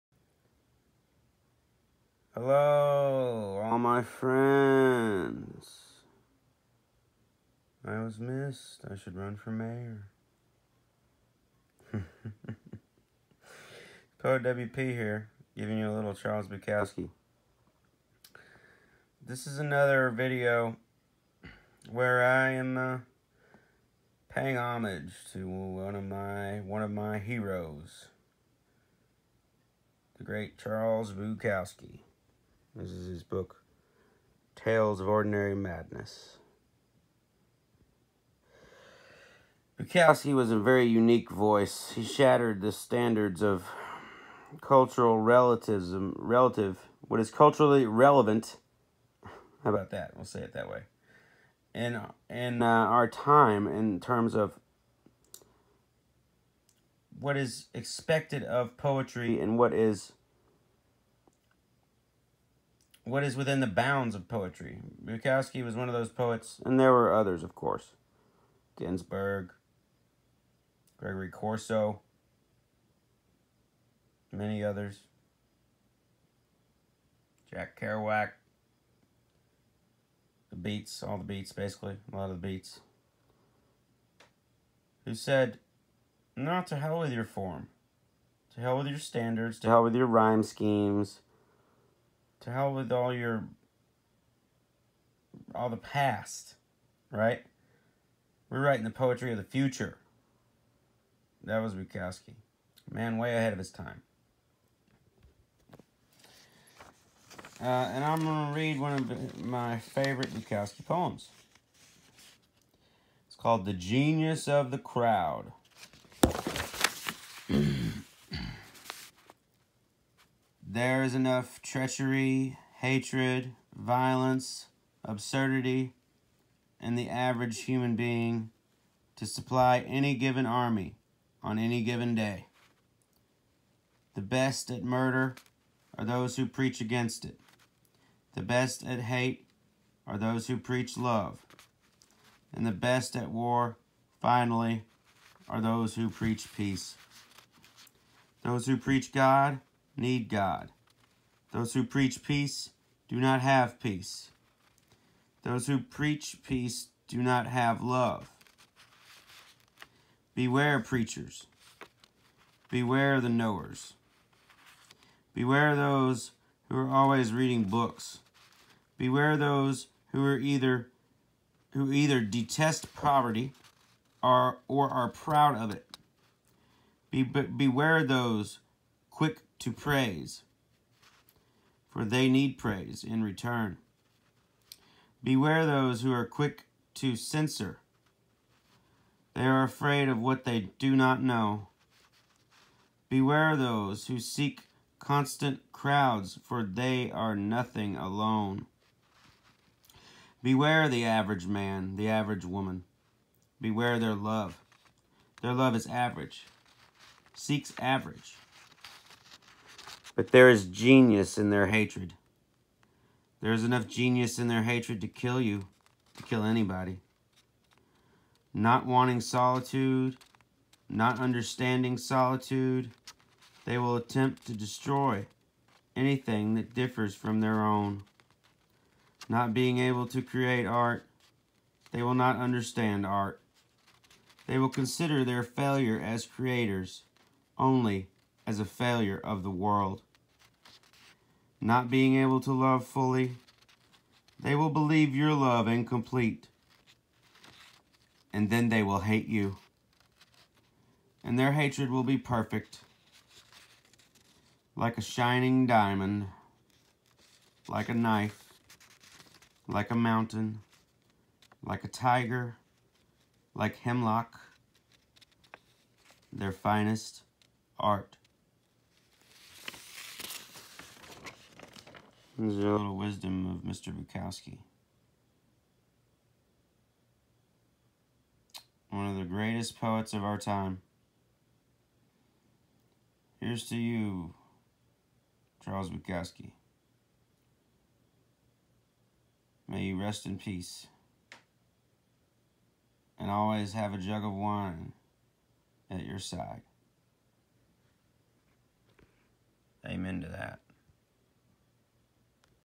A homage to Charles Bukowski (“don’t try”)🥊
In this vid I send out a dedication to the great Charles Bukowski.
He was a good duker 🥊🥊🥊🥊🥊🥊🥊